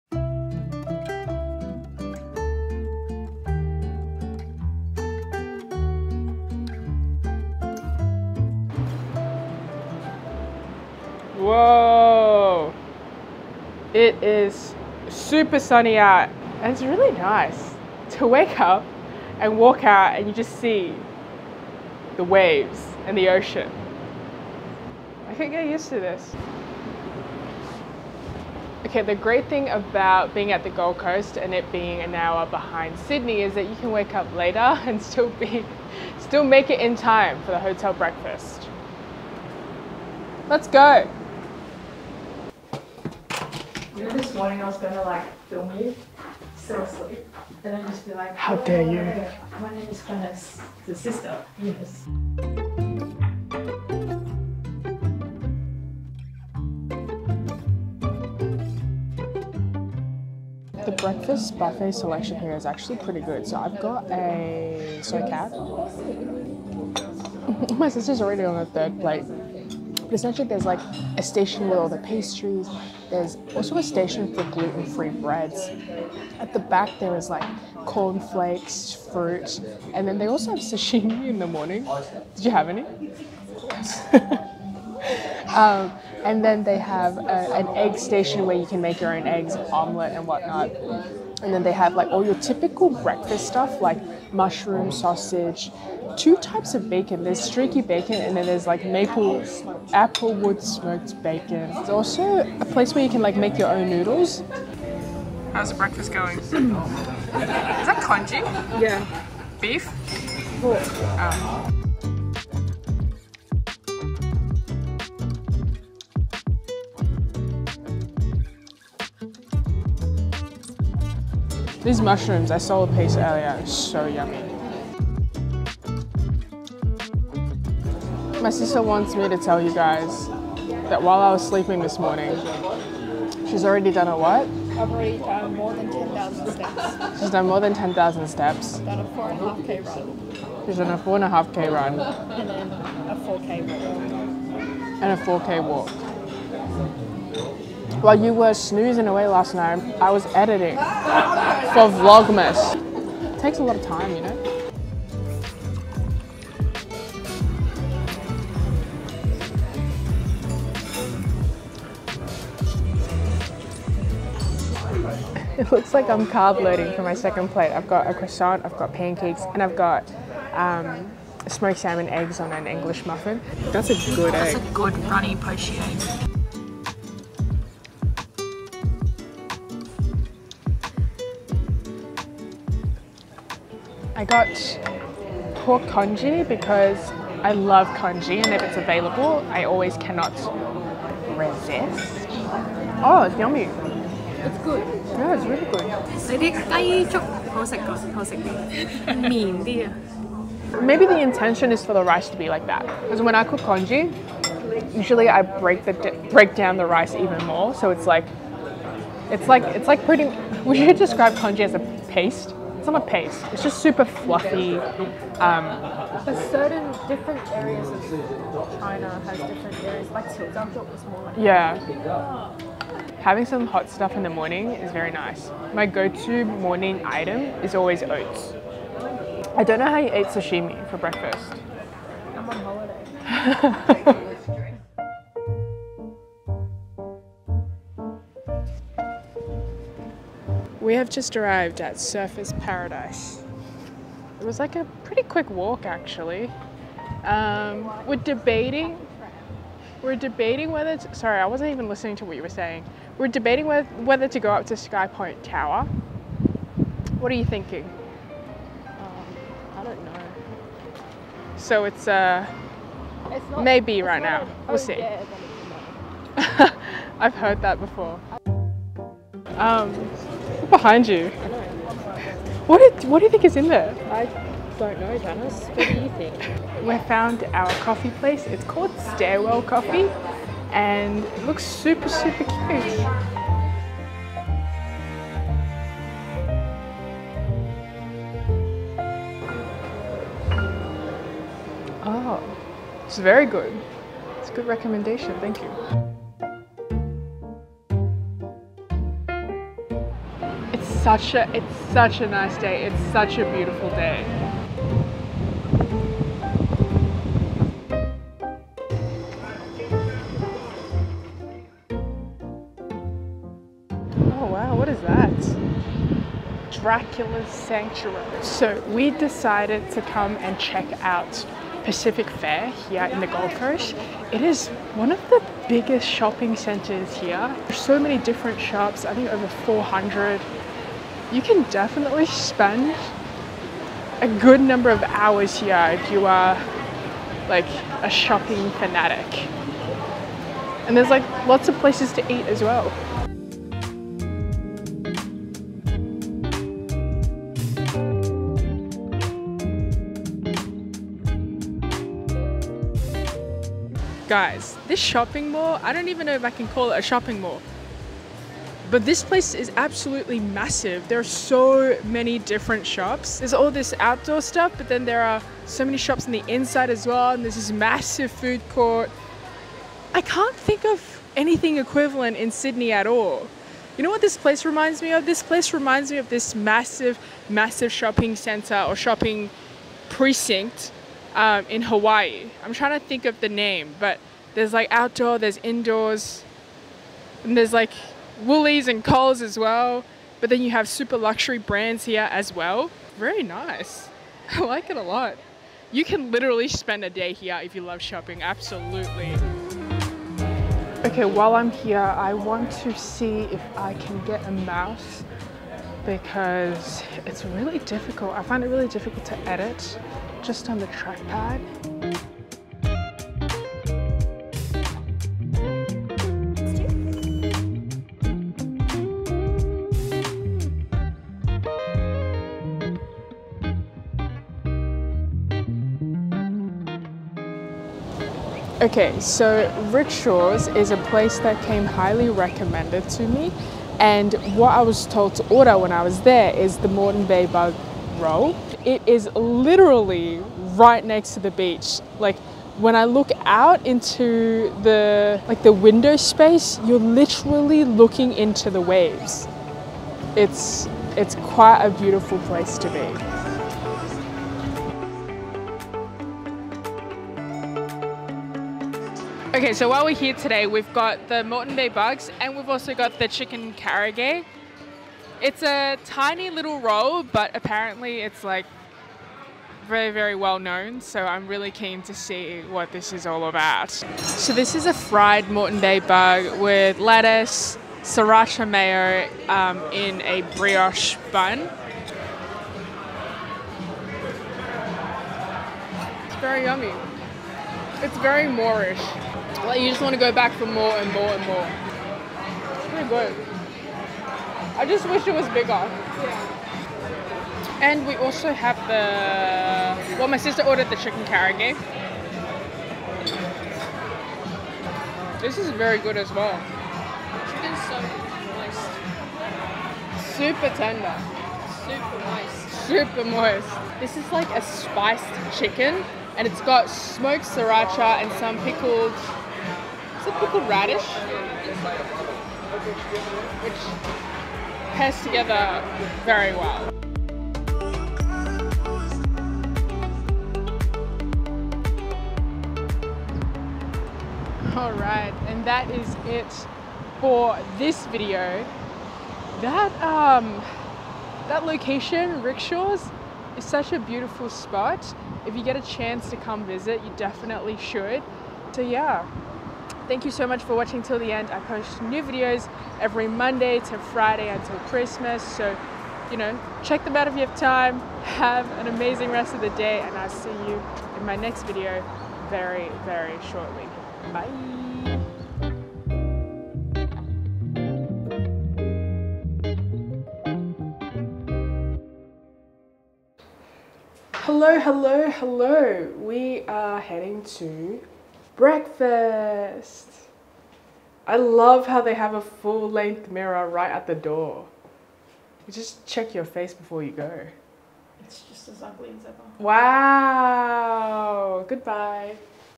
EPIC Breakfast Buffet, Coffee at Surfers Paradise & MUST VISIT Gold Coast Restaurant! Vlogmas 2022
Restaurants I visited:
1. Akoya - 38 Old Burleigh Rd, Surfers Paradise QLD 4217
    - Breakfast buffet
2. Stairwell Coffee - Shop 67/3131 Surfers Paradise Blvd, Surfers Paradise QLD 4217
    - Soy cap
3. Rick Shores - 3/43 Goodwin Terrace, Burleigh Heads QLD 4220
    - Molten bay bug roll
    - Chicken karaage

It’s the first full day in the Gold Coast, and we started it with a really satisfying breakfast buffet at the hotel, because as my sister says it best - “I’m on holiday”.

It was a chill day for the most part and got to visit 2 recommended eateries which both turned out delicious. So thank you. (you know who you are)

00:00 Intro
00:44 Breakfast buffet
04:36 Coffee stop to start the day
05:20 The Biggest Shopping Centre Ever!
07:10 A meal with the best view